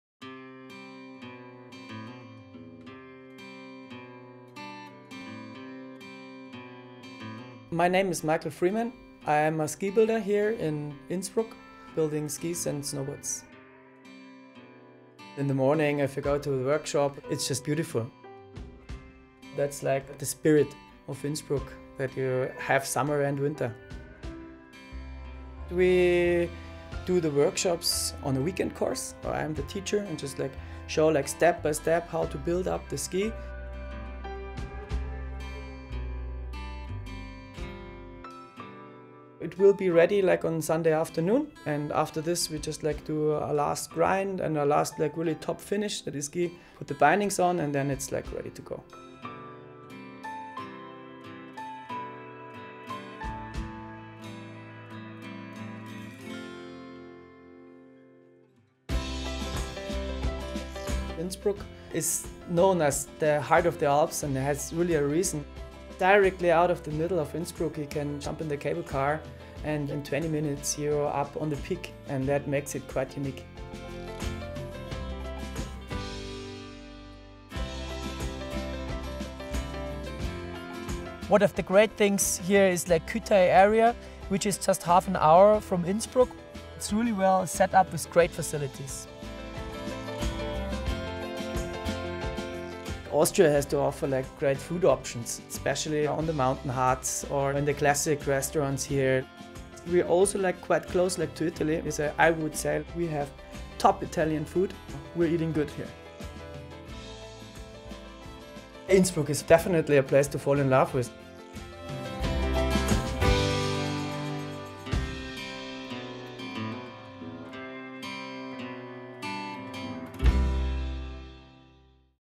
WinterTales: A Perfect Day in Innsbruck,Austria
Michael Freymann, an Innsbruck ski & snowboard maker, shares his passion for his stunning hometown and talks about the advantages of living close to so many ski areas in the Tirol.  Innsbruck is a unique location for skiing. The funicular only takes 20 minutes to get up to Nordkette, the ski area above the city.
Where will your heart take you?